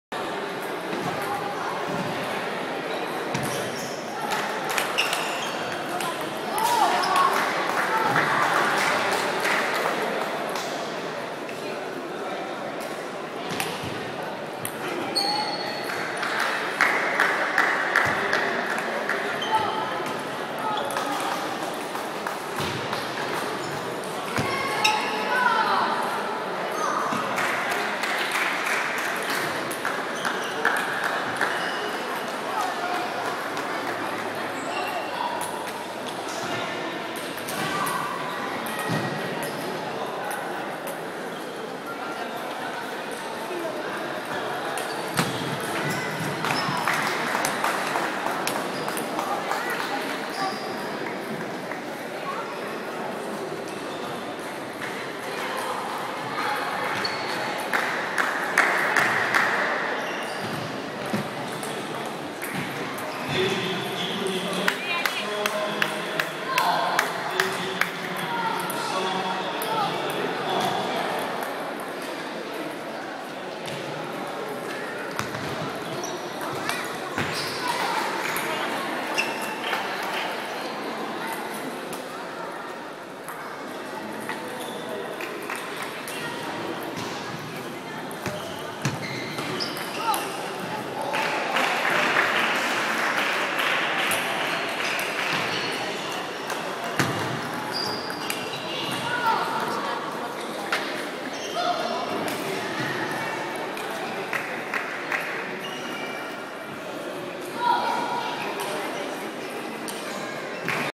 Eurominichamp_nathan_armenien.MOV
Match de Nathan Daviet en Poule quelques échanges